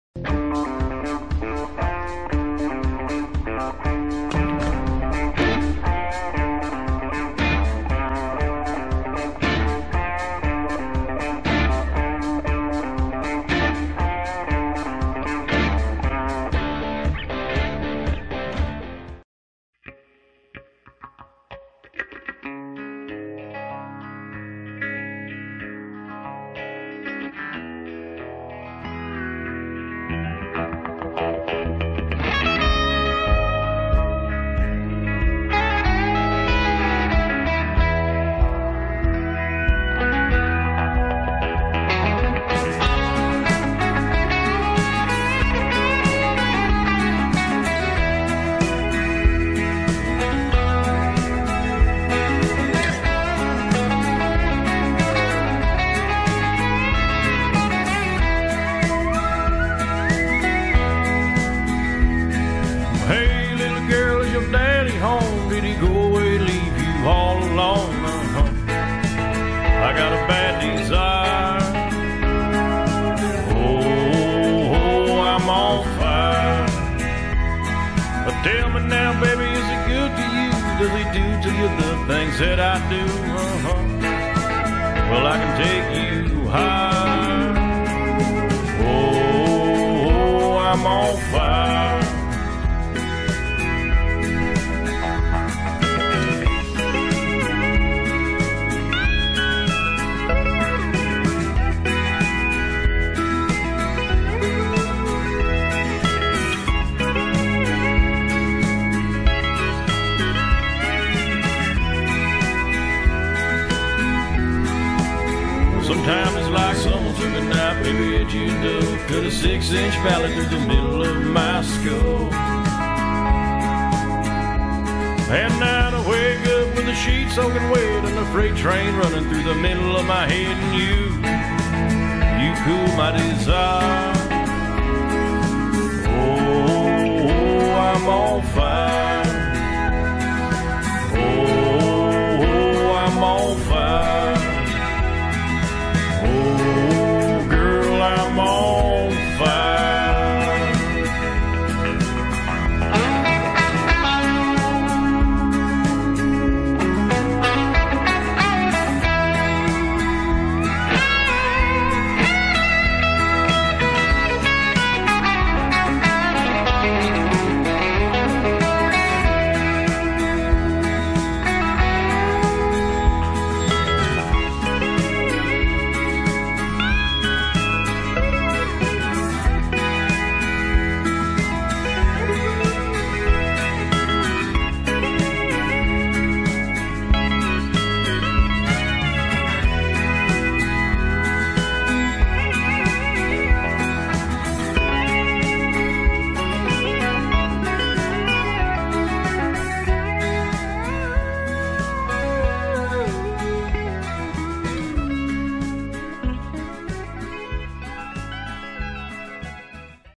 Drive up Whitney Portal Road to Whitney Portal
I was able to drive up the newly finished Whitney Portal road to Whitney Portal the day it was opened. Thought you guys would enjoy the views of the road.  What was a 15 minute drive has been compressed into 3 minutes.